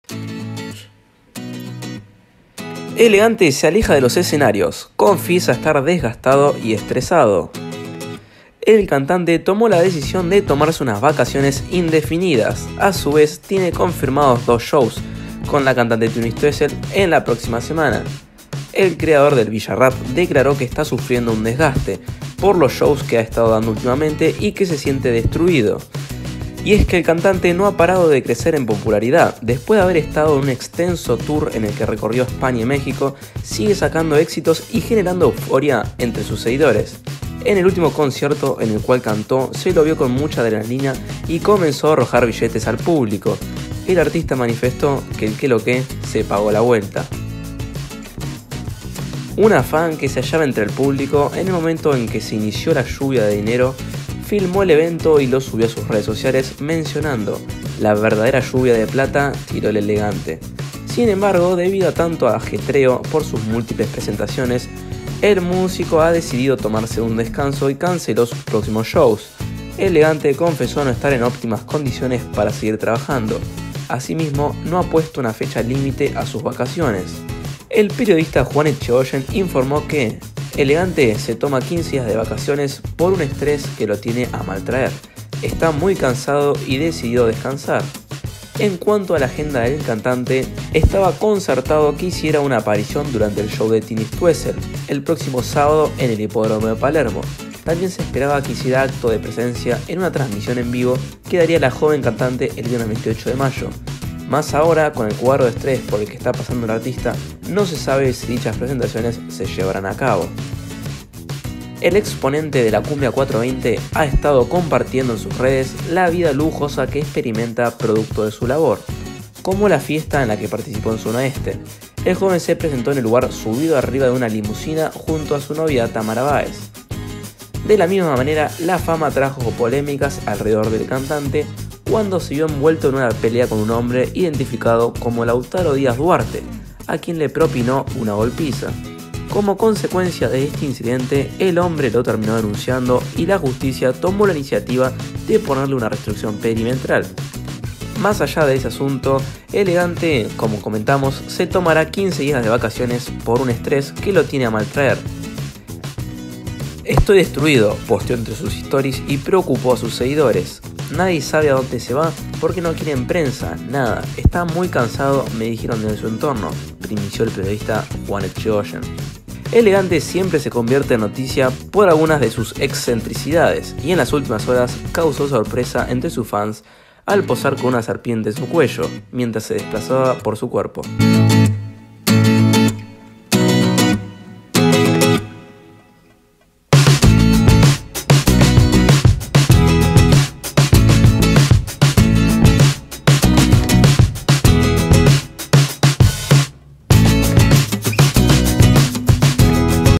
L-Gante se toma vacaciones confiesa estar desgastado y estresado
El cantante urbano hizo una confesión muy importante sobre su carrera